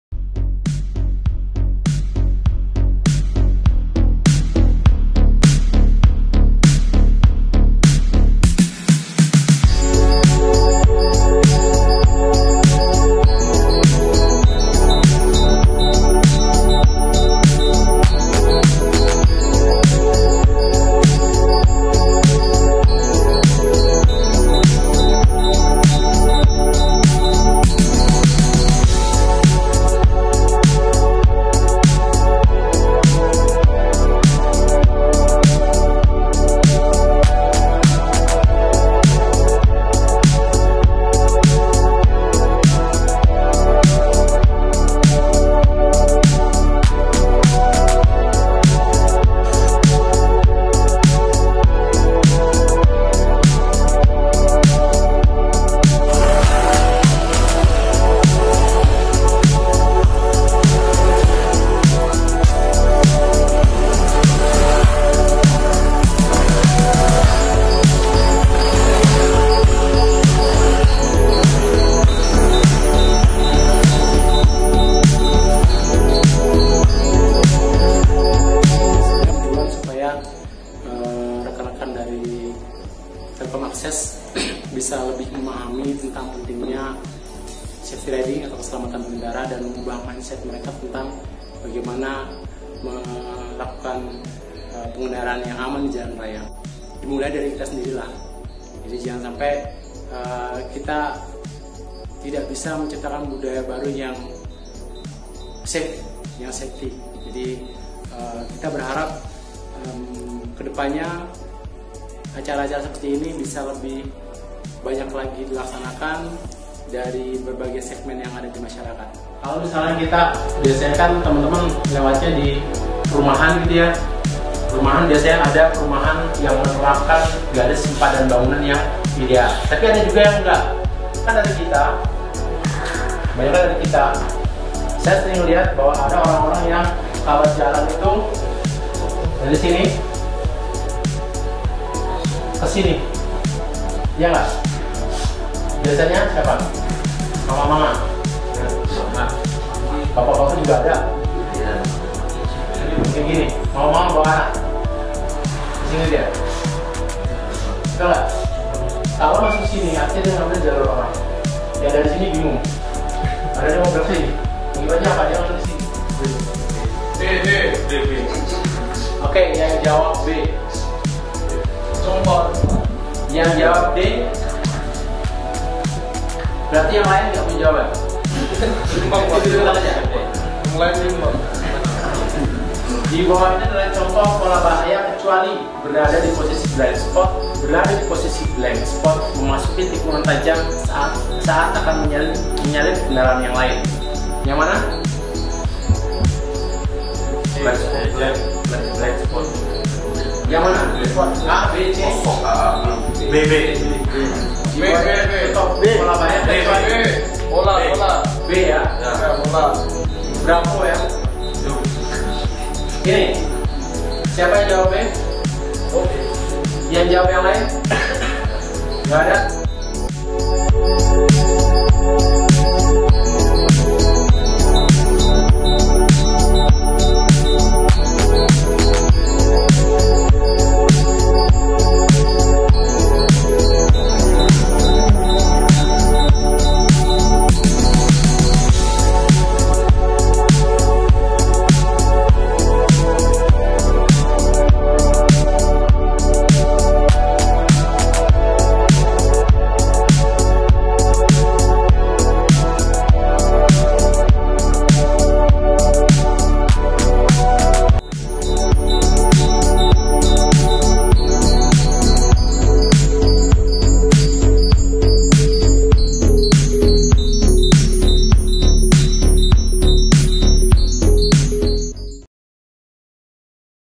SAFETY RIDING TELKOM AKSES BATAM 2020 BERSAMA HONDA CAPPELLA GROUP
Kegiatan ini Menjadi Dasar untuk mendukung Bulan K3 Nasional dan Pelopor Keselamatan Berkendara di Jalan Raya.